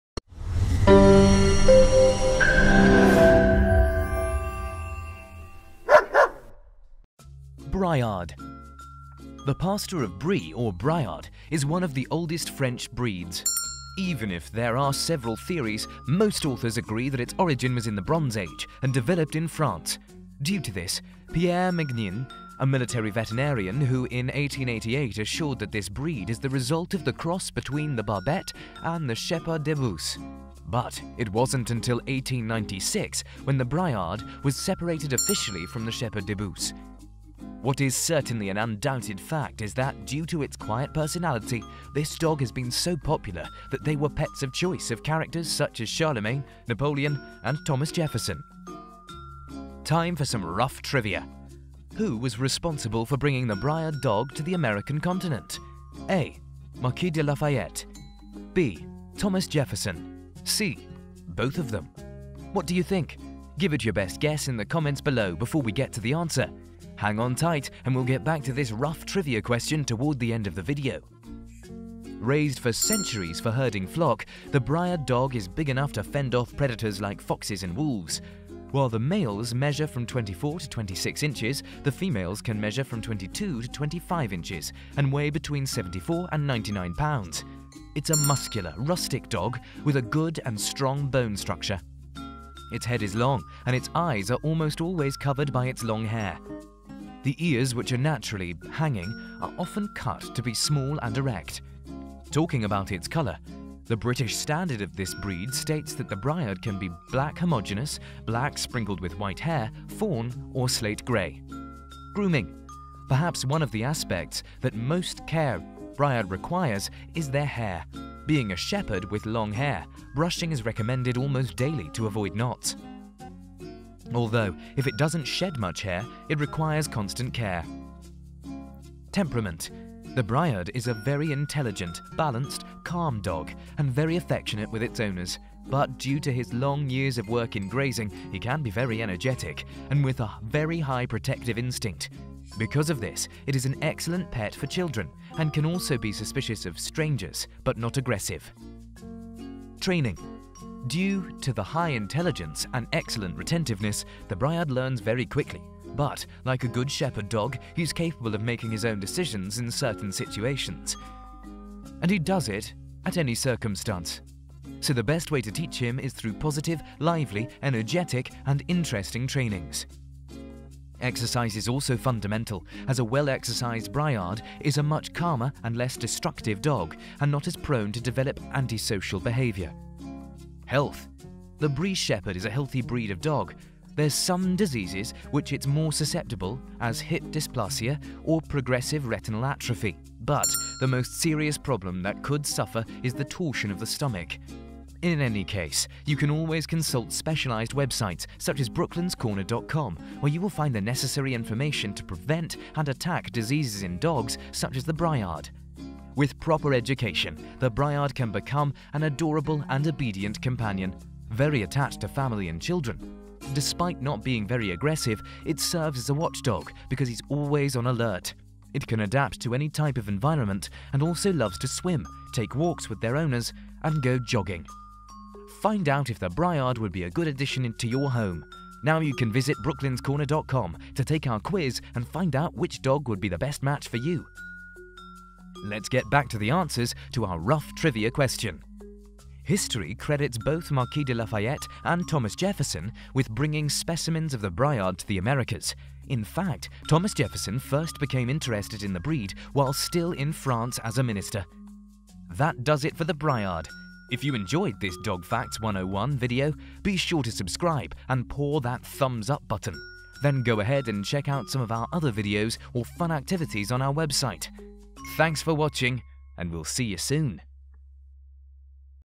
Dogs 101 - BRIARD - Top Dog Facts About the BRIARD
Briard
The Pastor of Brie or Briard is one of the oldest French breeds. Even if there are several theories, most authors agree that its origin was in the Bronze Age and developed in France. Due to this, Pierre Mégnin, a military veterinarian who in 1888 assured that this breed is the result of the cross between the Barbet and the Shepherd de Beauce. But, it wasn’t until 1896 when the Briard was separated officially from the Sheperd de Beauce. What is certainly an undoubted fact is that, due to its quiet personality, this dog has been so popular that they were pets of choice of characters such as Charlemagne, Napoleon and Thomas Jefferson.

Time for some Ruff Trivia:
- Who was responsible for bringing the Briard dog to the American continent?
o A:  Marquis De Lafayette
o B:  Thomas Jefferson
o C:  Both of them

What do you think, give it your best guess in the comments below before we get to the answer! Hang on tight and we’ll get back to this Ruff Trivia Question toward the end of the video.
Raised for centuries for herding  flock, the Briard dog is big enough to fend off predators like foxes and wolves. While the males measure from 24 to 26 inches, the females can measure from 22 to 25 inches and weigh between 74 and 99 pounds It’s a muscular, rustic dog, with a good and strong bone structure. His head is long and his eyes are almost always covered by his long hair. The ears, which are naturally hanging, are often cut to be small and erect. Talking about it’s color, The British standard of this breed states that the Briard can be black homogeneous, black sprinkled with white hair, fawn or slate gray.

Grooming:

Perhaps one of the aspects that most care Briard requires is their hair, being a shepherd with long hair, brushing is recommended almost daily to avoid knots. Although if it doesn’t shed much hair, it requires constant care.

Temperament:

The Briard is a very intelligent, balanced, calm dog and very affectionate with its owners. But, due to his long years of work in grazing, he can be very energetic and with a very high protective instinct, because of this it is an excellent pet to children and can also be suspicious of strangers, but not aggressive.

Training:
Due of his great intelligence and excellent retentiveness, the Briard learns very quickly. But, like a good shepherd dog, he’s capable of make his own decisions in certain situations, and he do it at any circumstance; so the best way to teach him is through positive, lively, energetic and interesting trainings. Exercise is also fundamental, as a well-exercised Briard is a much calmer and less destructive dog, and not as prone to develop antisocial behavior.

Health:
The Brie Shepherd is a healthy breed of dog, there’re some diseases which its more susceptible as hip dysplasia or progressive retinal atrophy. But, the most serious problem that could suffer is the torsion of the stomach. In any case, you can always consult specialized websites such as BrooklynsCorner.com, where you will find the necessary information to prevent and attack diseases in dogs such as the Briard.

With proper education, the Briard can become in an adorable and obedient companion, very attached to family and children. Despite not being very aggressive, it serves as a watchdog because he’s always on alert. It can adapt to any type of environment and also loves to swim, take walks with their owners and go jogging.